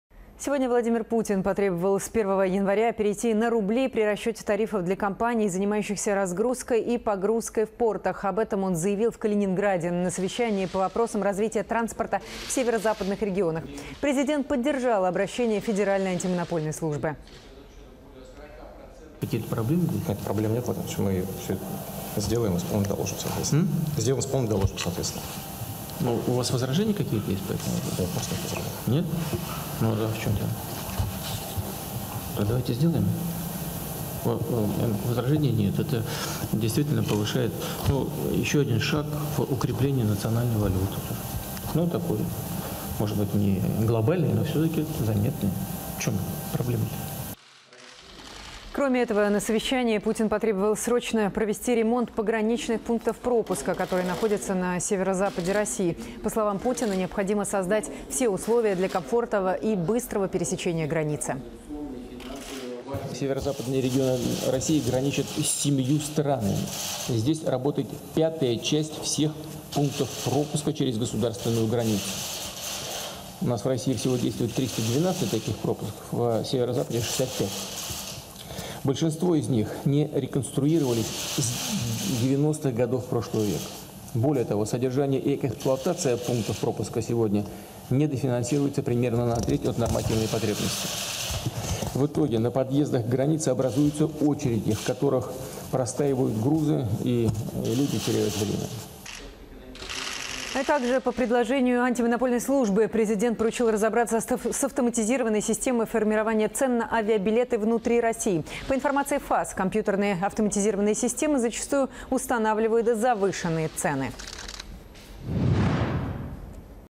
Путин поручил перейти на рубли в тарифах на разгрузку в портах
Владимир Путин потребовал с 1 января перейти на рубли при расчете тарифов для компаний, занимающихся разгрузкой и погрузкой в портах.